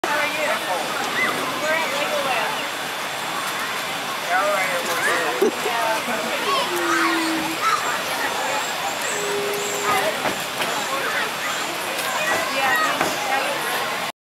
Kyle's Funny Family Dryer Dance at Legoland, CA December 2011
The family was drying off in an outside dryer after getting soaked on one of the water rides, and Kyle thought it would be a good time to do an impromptu dance. He was 11 years old, and actually hates music, lol, but sometimes just feels like dancing. 8 years later, he still does this kind of stuff all the time. He'll just break into a dance in the middle of the grocery store sometimes. Perhaps he hates music because it interferes with the music that's already in his head!? Unfortunately, I was only able to get my camera out in time to catch just the last few seconds of this one.